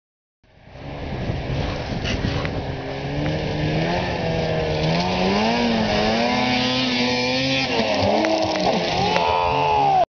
Jumps on 600 mxz
out buchans flying jumps on ski-doos